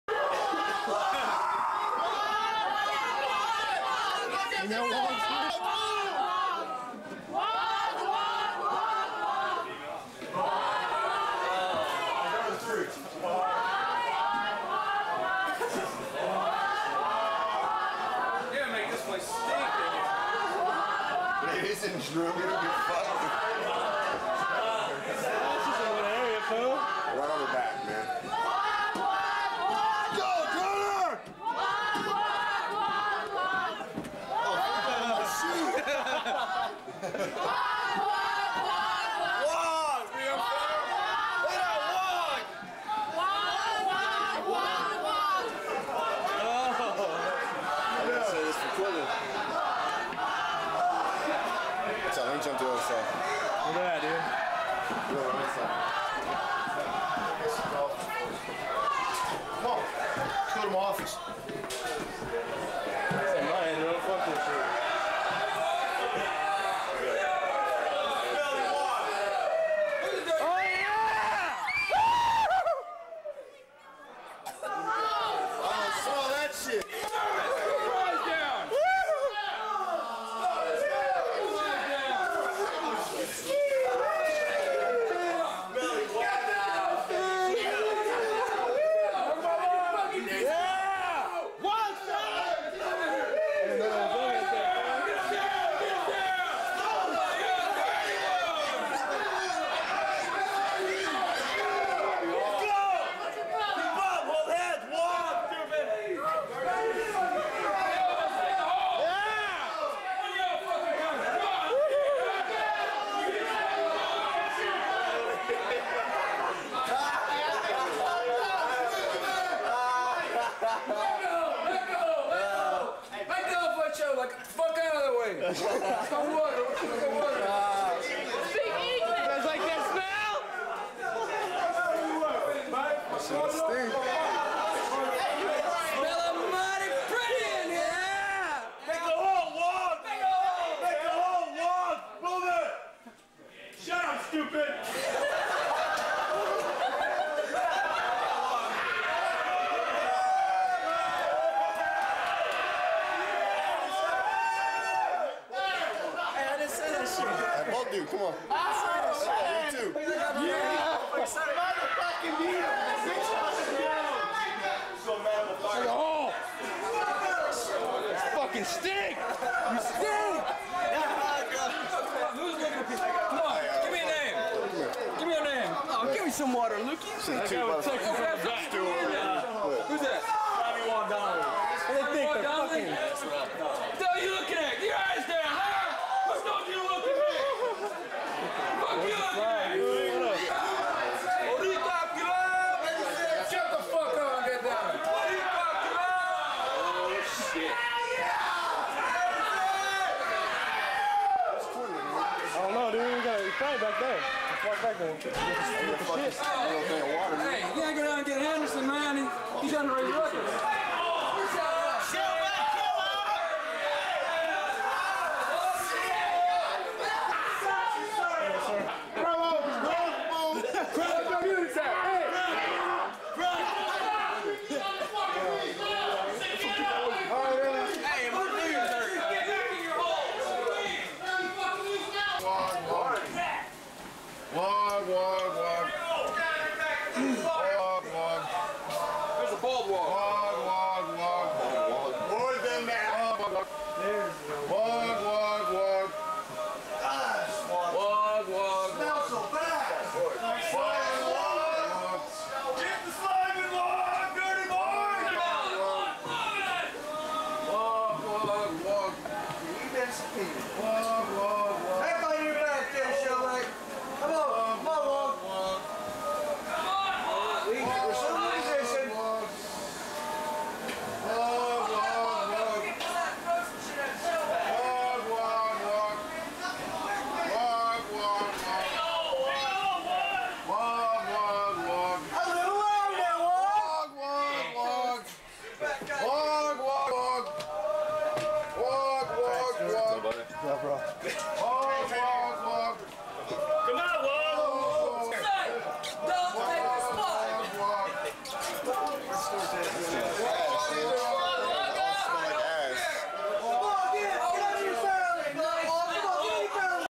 USS Abraham Lincoln West Pac 2000-2001 'WOG DAY'
Golden Shellback beginnings USS Abraham Lincoln's Wog Day during West Pac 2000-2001.